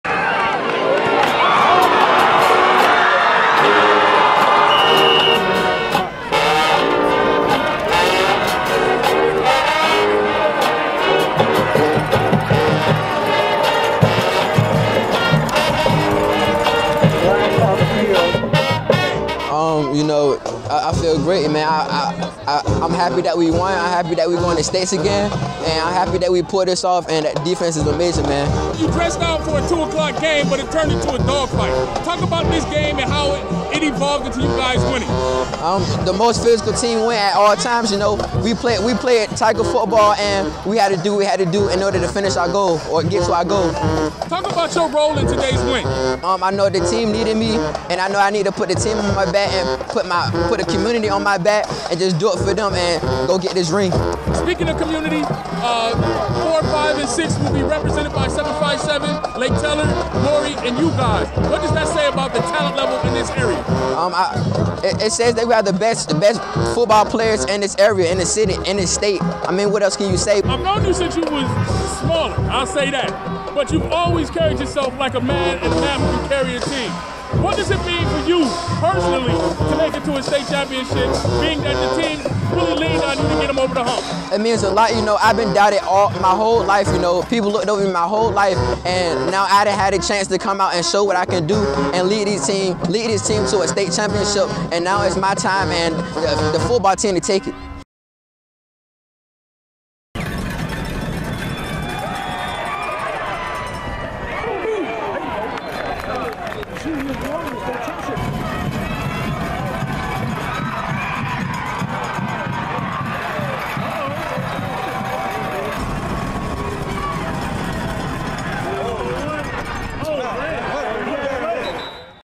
Romon Copeland after Oscar Smith's 18-16 state semifinal win over Colonial Forge
Game highlights and post-game interview with Romon "Man Man" Copeland after Oscar Smith's 18=16 win over Colonial Forge.  Video by Antowyne Shaw/recruit757.